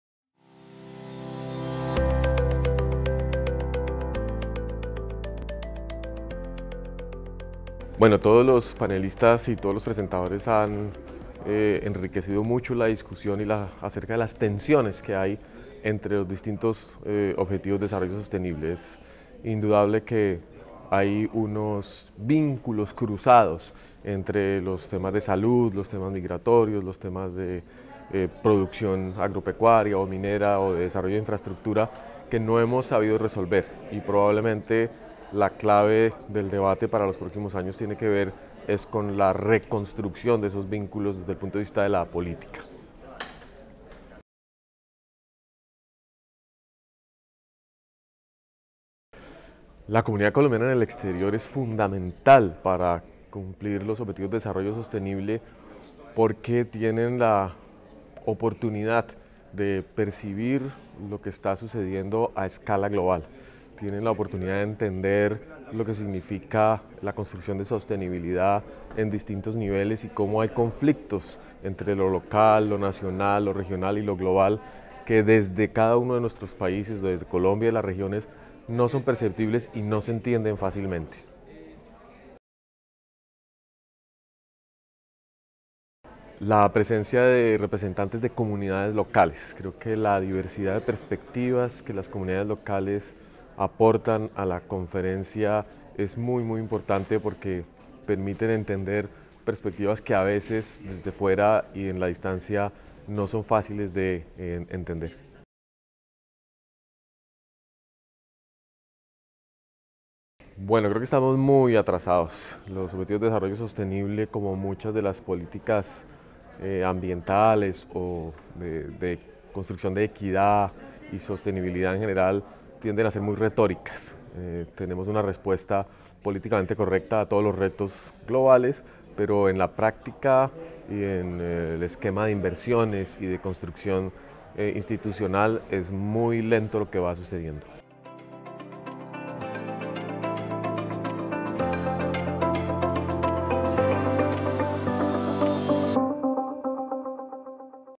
Shaping Colombia 2019, entrevista con Brigitte Baptiste
Colombian Conference, Shaping Colombia 2019

Entrevista con Brigitte Baptiste, General Director of Instituto Humboldt